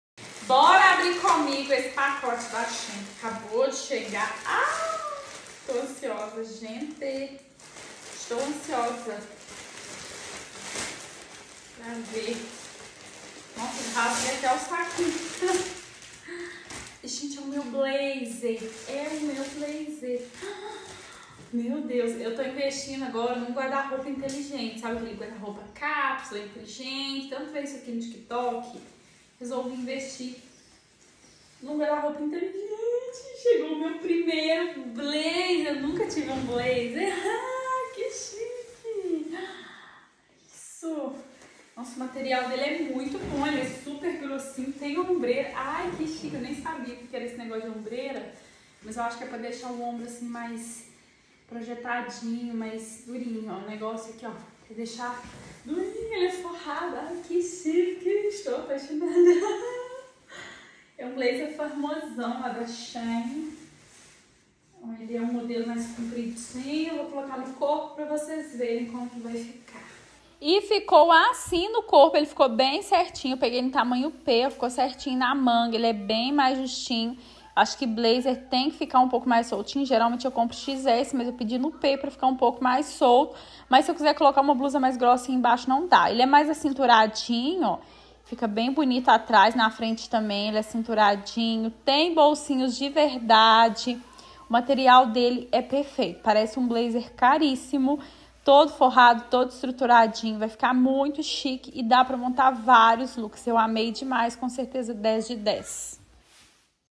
O Blazer preto mais elegante da SHEIN
Id: 4388651 me conta aqui oque acharam? Eu estou mudando meu estilo pra ver se fico mais elegante kkkk nesse vídeo ainda não estou elegante porq agora que as roupas começaram a chegar 😅 se fosse arrumar cabelo e fazer maquiagem não fazia o vídeo pois aproveitei o sono da bebê pra gravar então relevem é só uma fase!